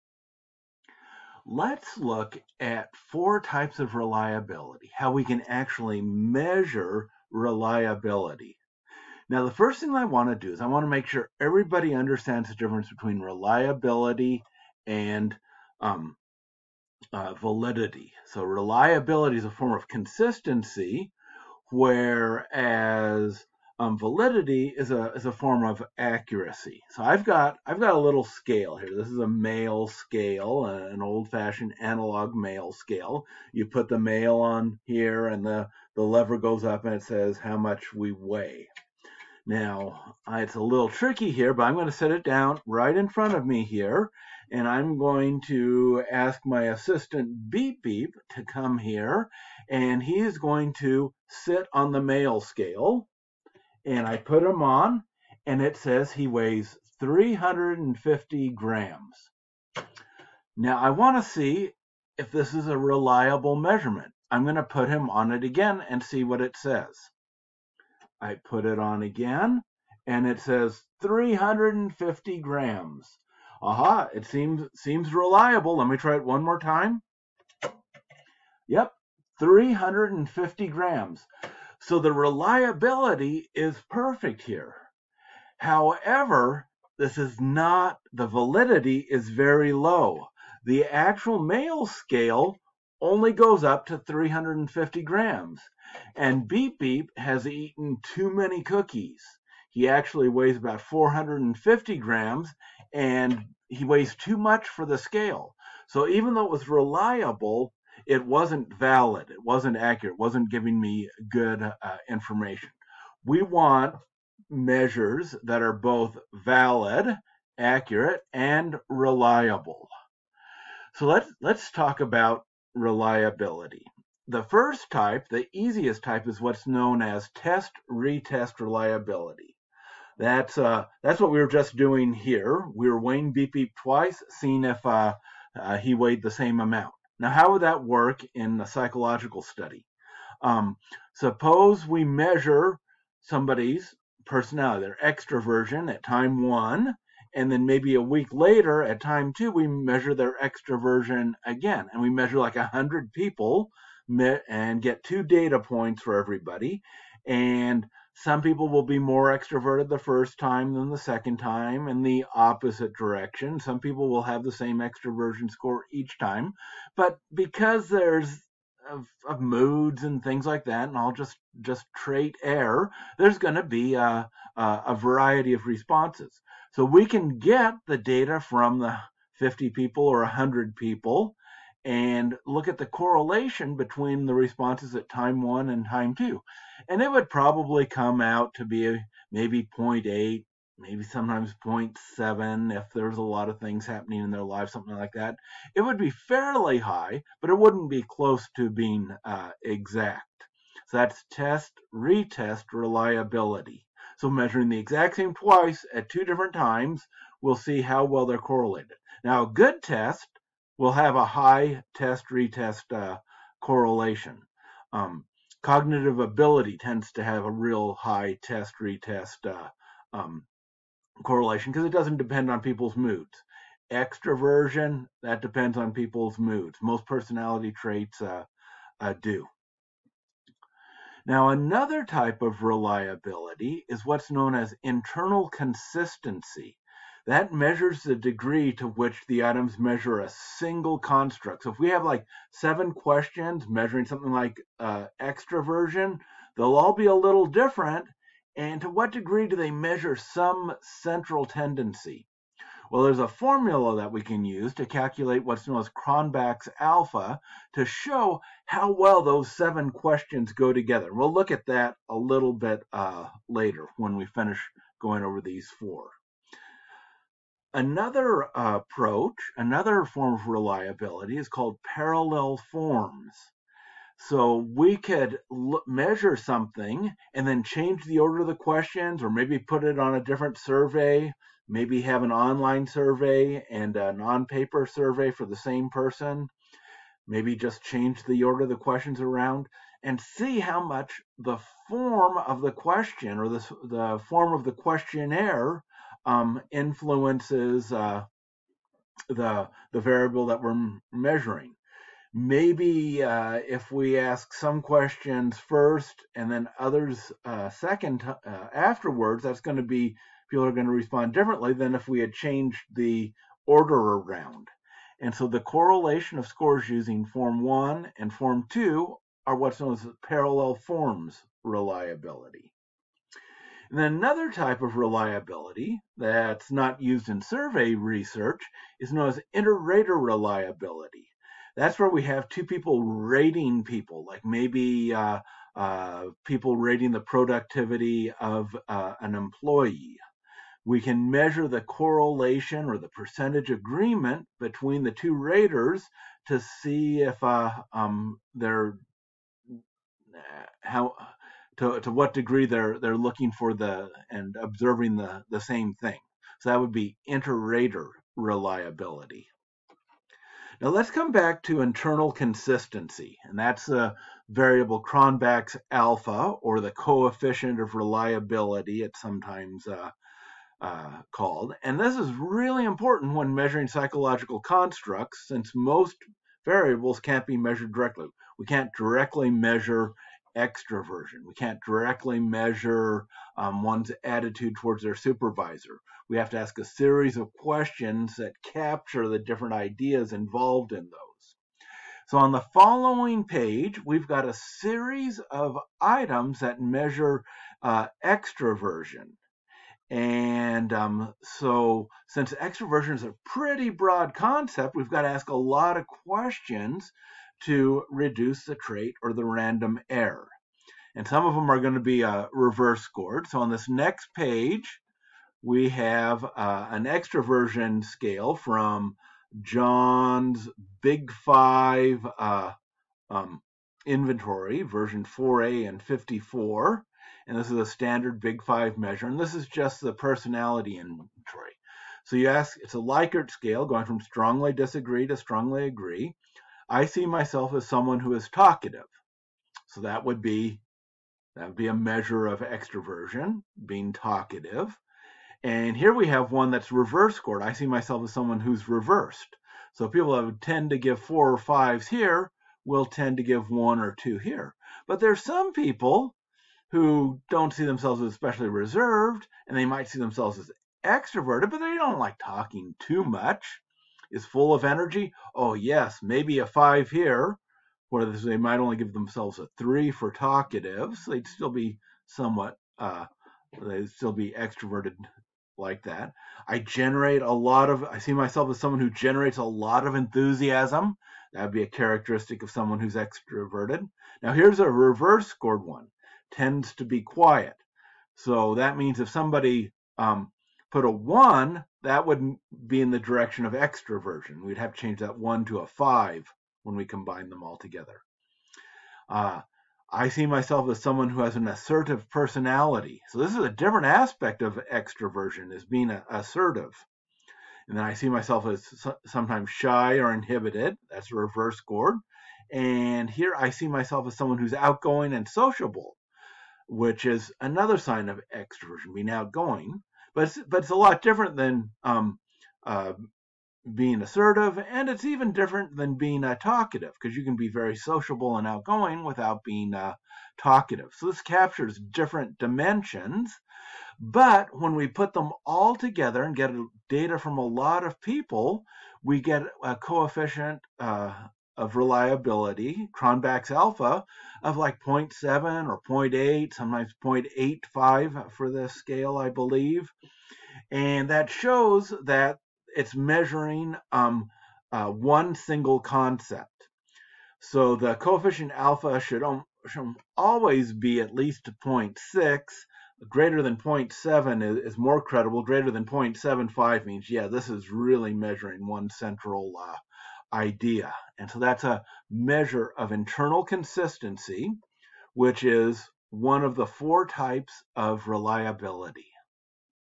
Four Types of Reliability: Test-Retest, Internal Consistency, Parallel Forms, and Inter-Rater
This video discusses 4 types of reliability used in psychological research.
The text comes from from Research Methods and Survey Applications by David R. Dunaetz. You can find a free PDF of this book by googling the title or you can buy a hard copy on Amazon.